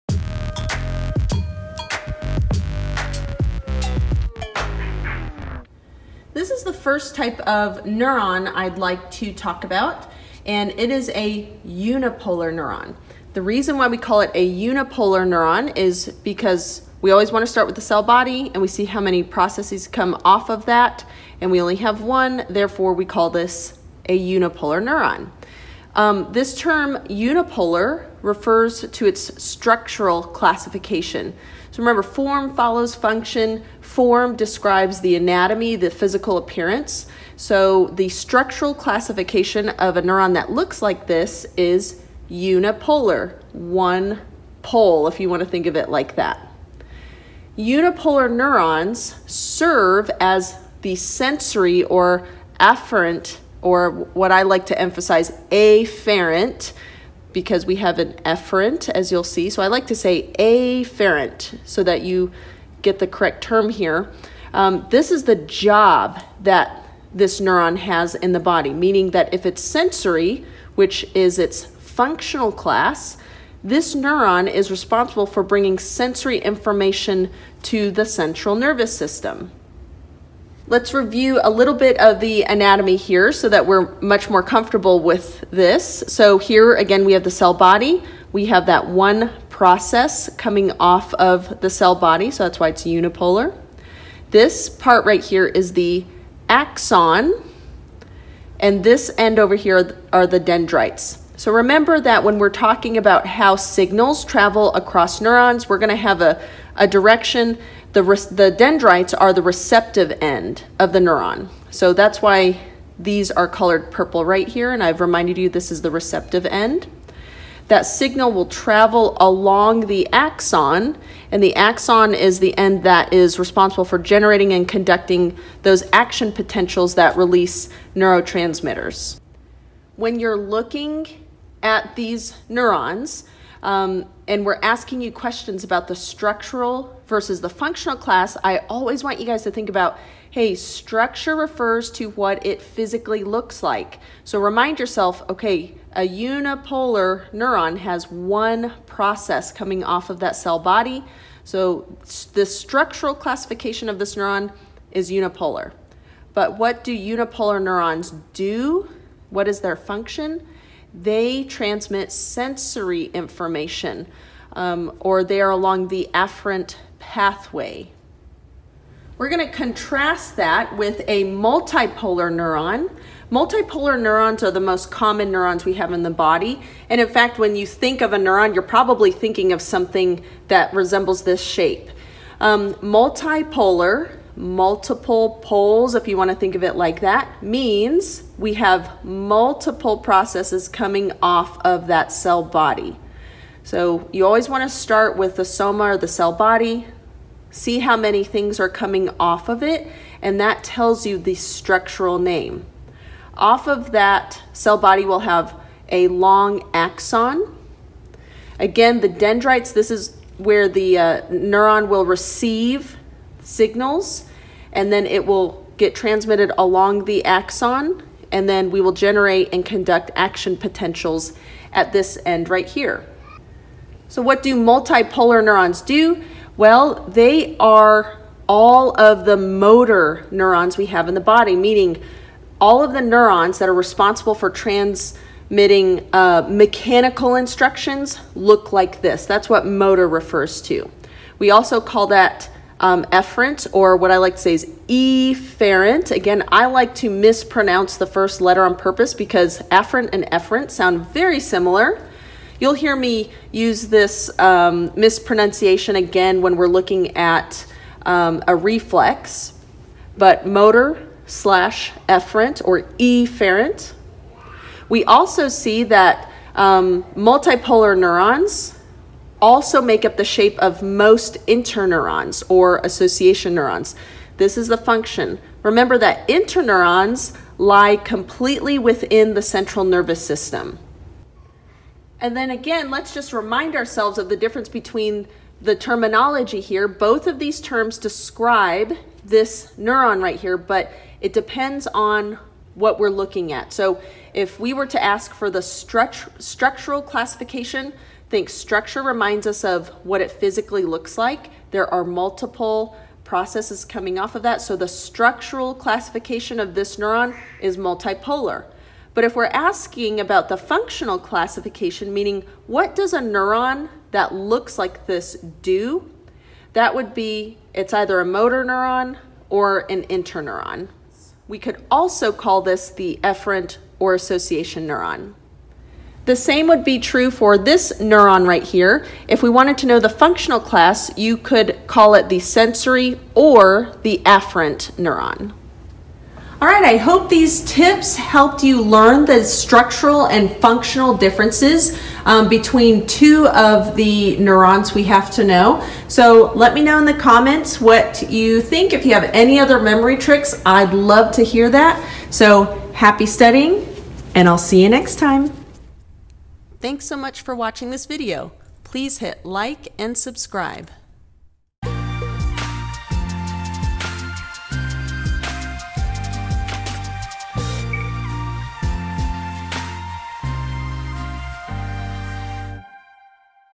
Neuron - Structural Versus Functional Classification
Here I discuss how we identify two types of neurons based on their physical appearance and the job(s) they perform.

Key Takeaways:
- Structural classification = name that describes the physical appearance
- Functional classification = name that describes the job of the neuron
- Unipolar = Sensory/Afferent
- Multipolar = Motor/Efferent